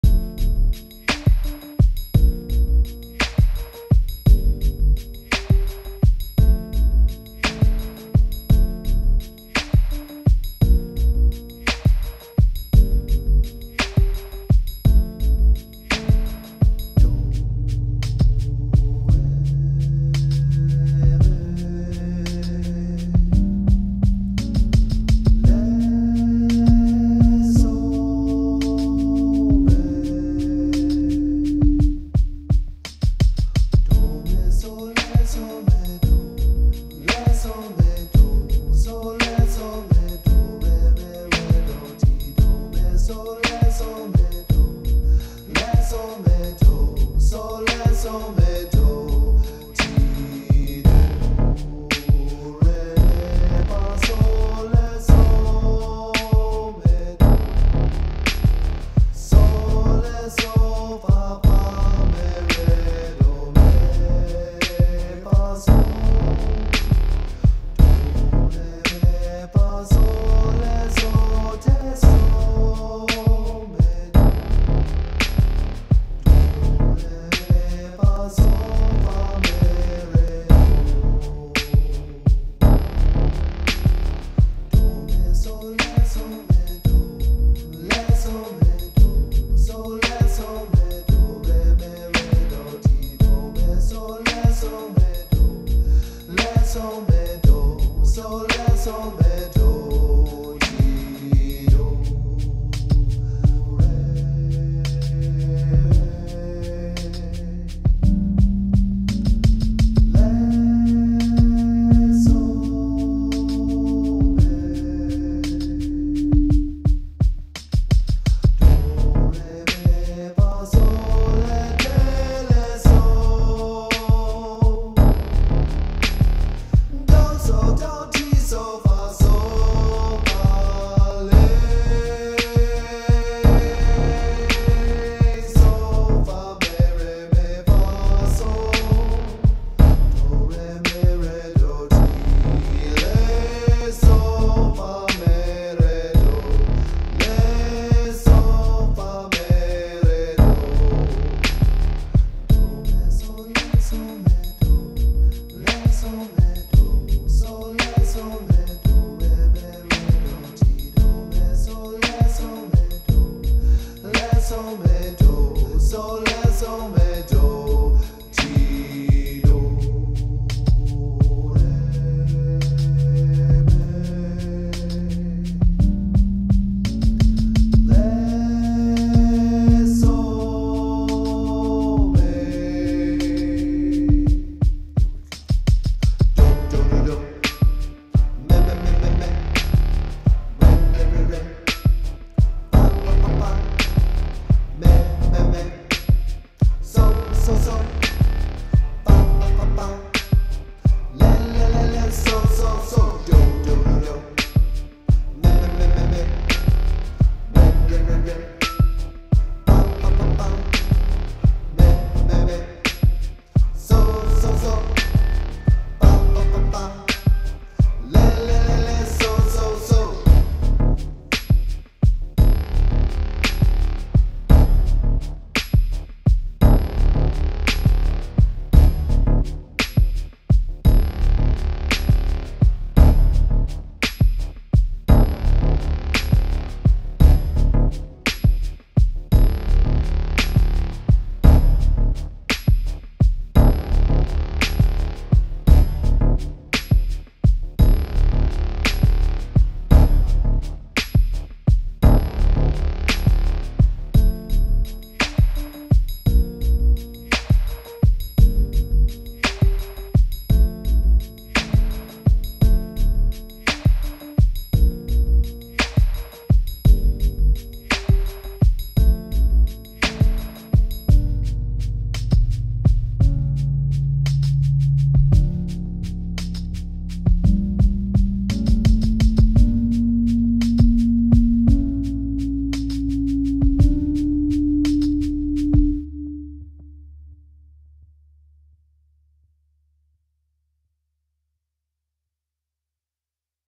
Singalong: Compound Minor
This video is about singing in minor in a compound meter. And determination